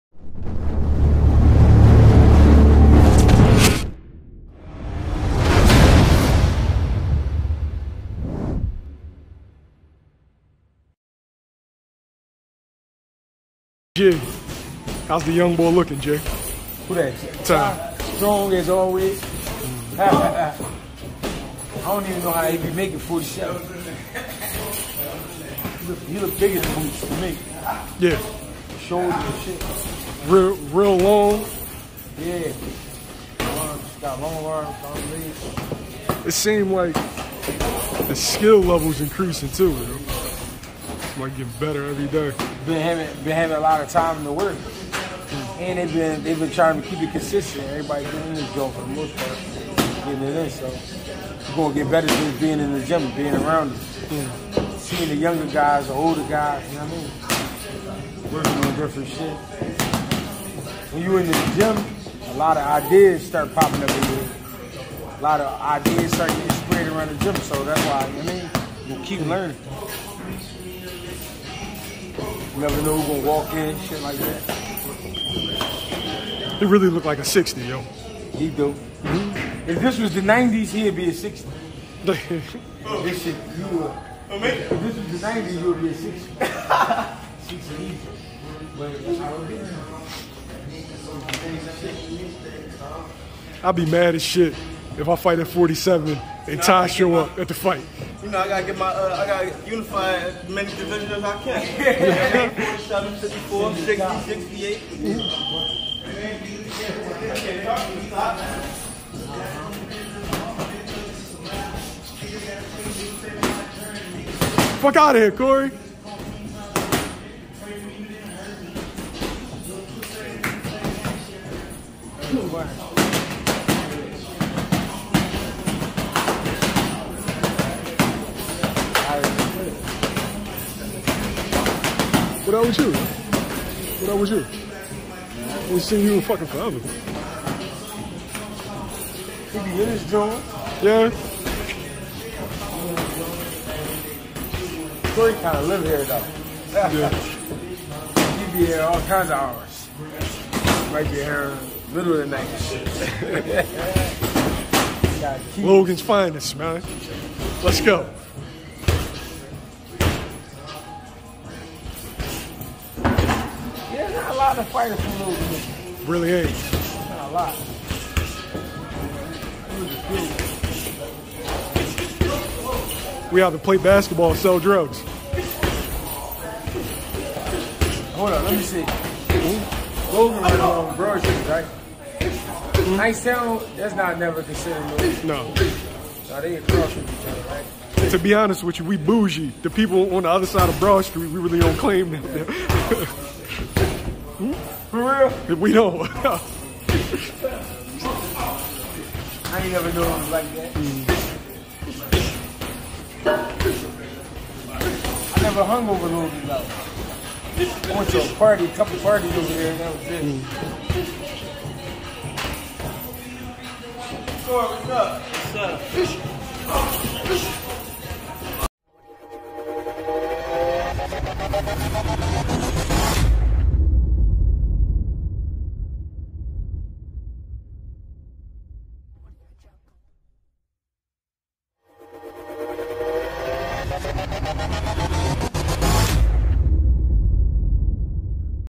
Greg Hackett breaks down the reason why Tahmir Smalls has started to peak
Trainer Greg Hackett watches Undefeated Welterweight Prospect Tahmir Smalls 8-0 5KO's sharpen his skills with his Father Lawrence Smalls at Philly 1on1 boxing gym.